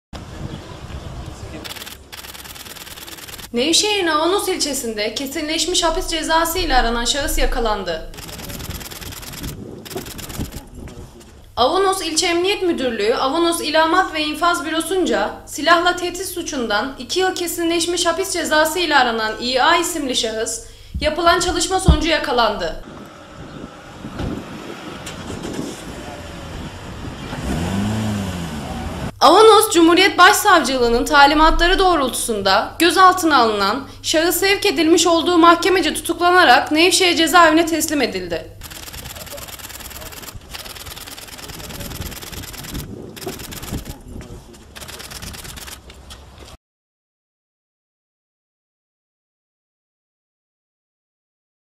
Avanos’ta Kesinleşmiş Hapis Cezası İle Aranan Şahıs Yakalandı
Anadolu RTV - Nevşehir Kent Haber

Avanos İlçe Emniyet Müdürlüğü Avanos İlamat ve İnfaz Bürosunca “SİLAHLA TEHDİT” suçundan 2 yıl KESİNLEŞMİŞ HAPİS CEZASI  ile aranan İ.A. (45) isimli şahıs yapılan çalışma sonucu yakalanmış,

Avcanos Cumhuriyet Başsavcılığının talimatları doğrultusunda gözaltına alınan şahıs sevk edilmiş olduğu Mahkemece TUTUKLANARAK ilimiz Cezaevine teslim edilmiştir.

GÜVENLİ ŞEHİR… NEVŞEHİR…